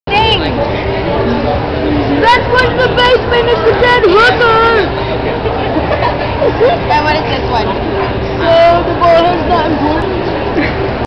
Laura Bailey as Shin Chan Acen 08
Laura Bailey doing Shin Chan's voice while signing my Shin sayings.